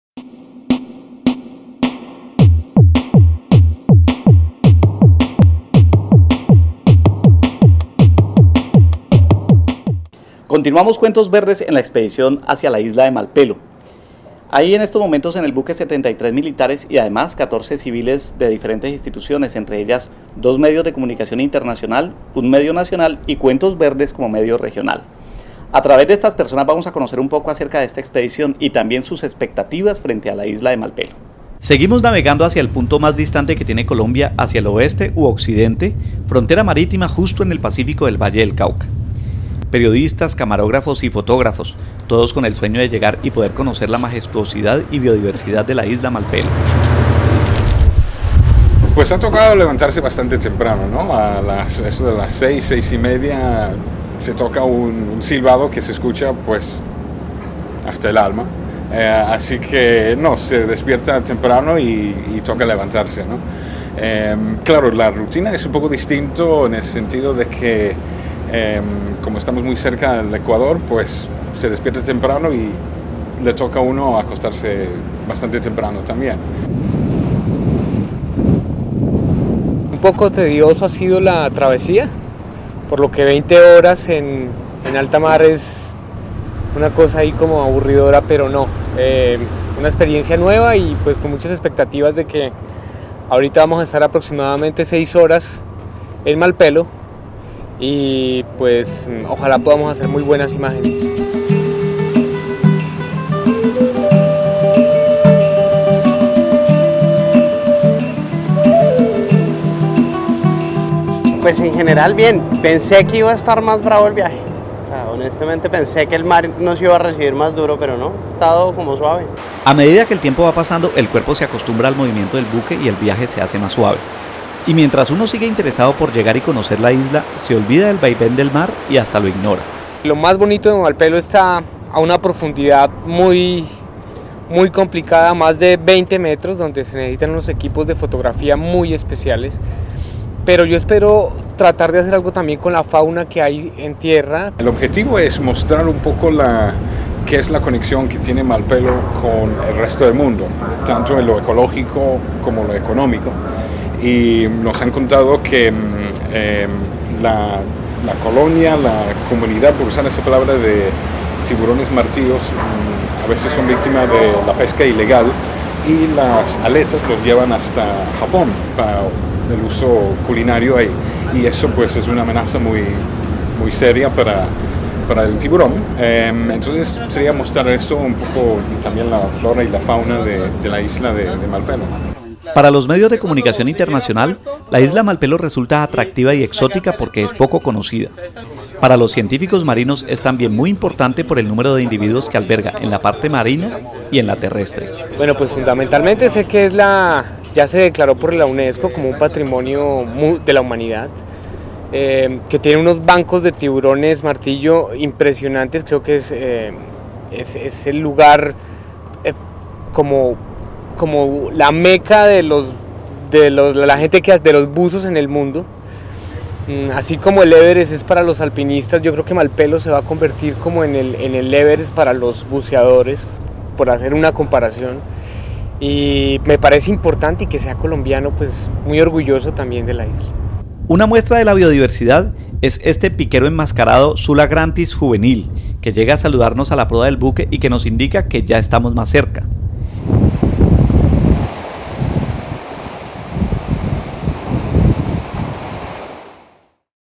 Expedición a la isla Malpelo 2 de 5
La expedición con Cuentos Verdes que realicé a la isla Malpelo en 2016, recién declarado el Parque Nacional como Patrimonio Natural de la Humanidad ante la UNESCO.